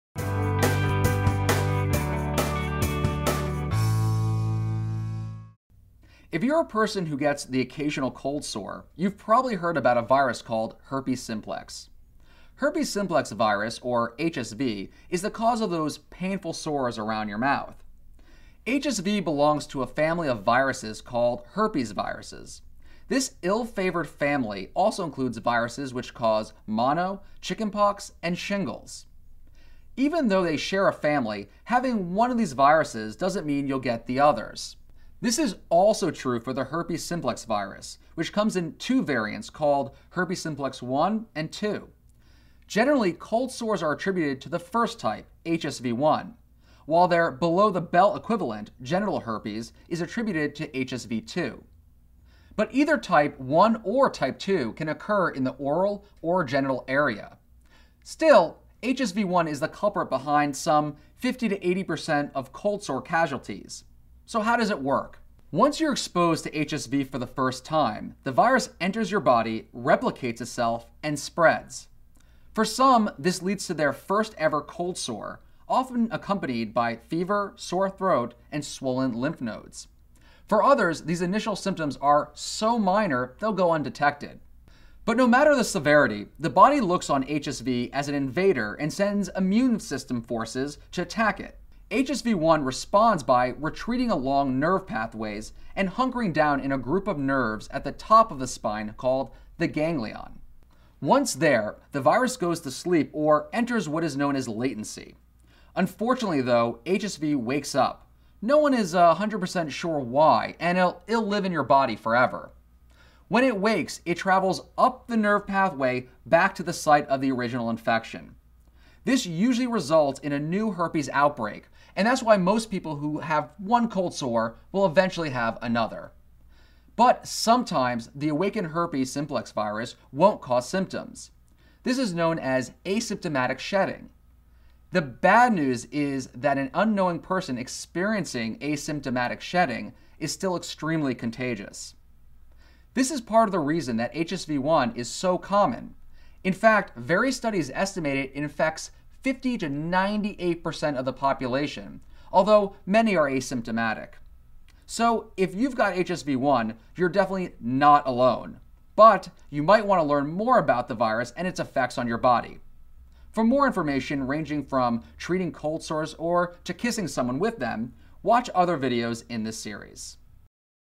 What is HSV-1? (Beauty & Grooming Guru)
While other viruses in the herpes family cause mono, chicken pox and shingles, HSV-1 causes cold sores.